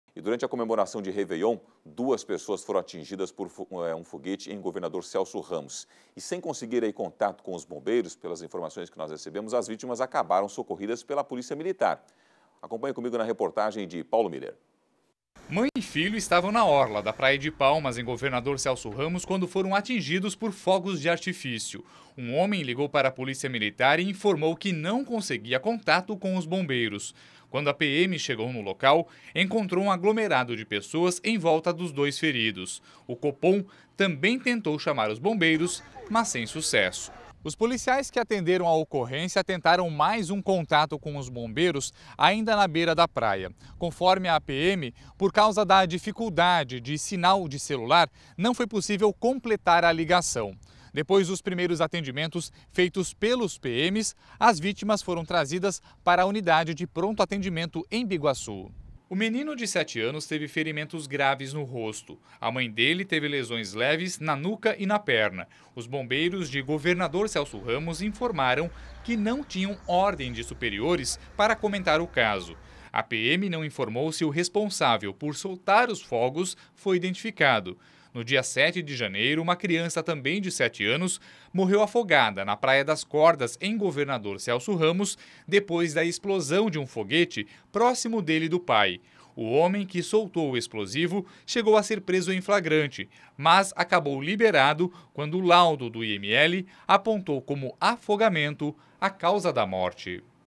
Mãe e filho ficam feridos ao serem atingidos por foguetes em Governador Celso Ramos
Durante a comemoração de réveillon, duas pessoas foram atingidas por foguetes em Governador Celso Ramos. Sem conseguir contar com os bombeiros, as vítimas acabaram socorridas pela Polícia Militar. Confira na reportagem.